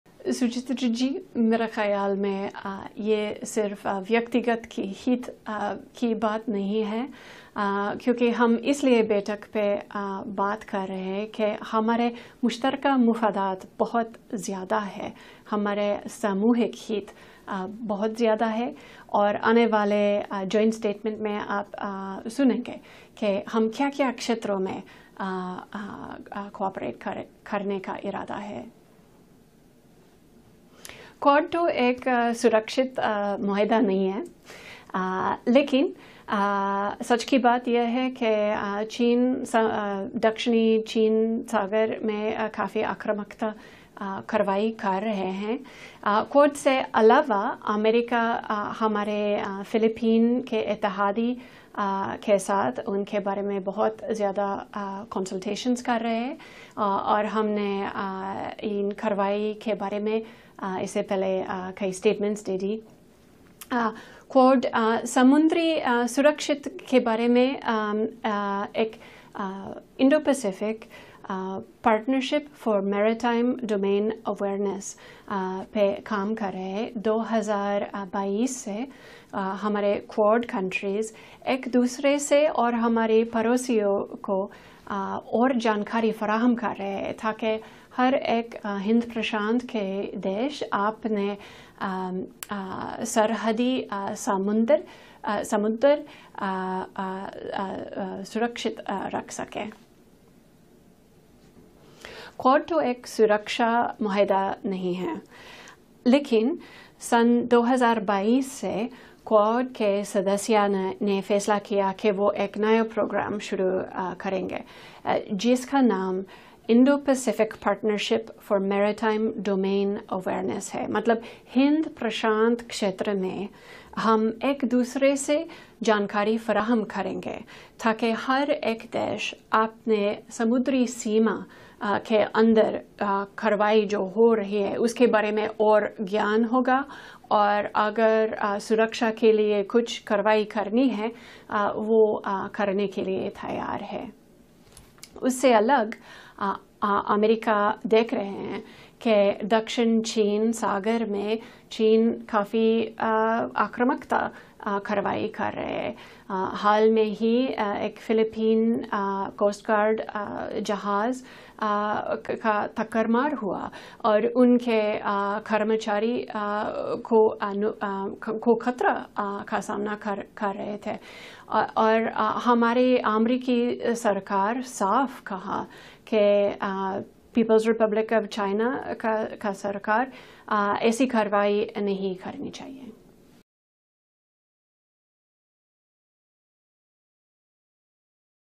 Margaret MacLeod’s Hindi Address on QUAD Summit: "Jabse Quad Bana Hai..."
US Spokesperson Margret Macleod outlined the key outcomes of the recent Quad Summit, highlighting collaborative efforts on regional security, climate change, and technology partnerships. She emphasized the importance of unity among member nations to address global challenges effectively.